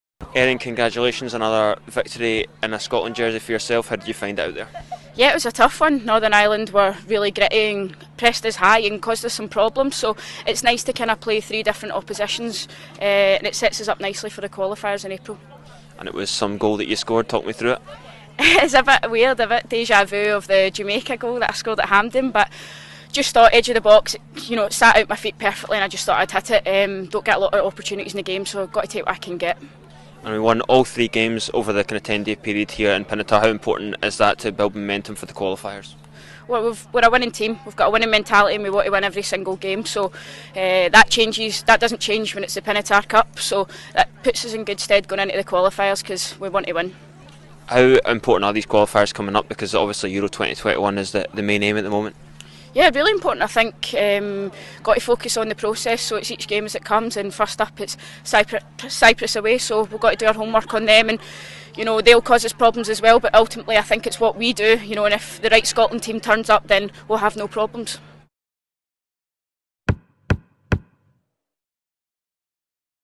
REACTION | Erin Cuthbert | Northern Ireland 1-2 Scotland | Pinatar Cup | SWNT
Erin Cuthbert reflects on Scotland's 2-1 victory over Northern Ireland in the Pinatar Cup.